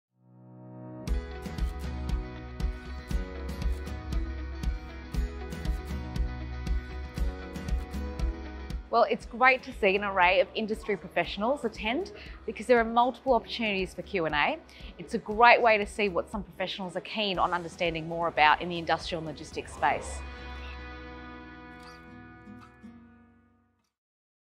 What do you enjoy most about being a Property Cpuncil Academy speaker?
Hear from Sass J-Baleh from CBRE on what she enjoys most about being a Property Council Academy speaker.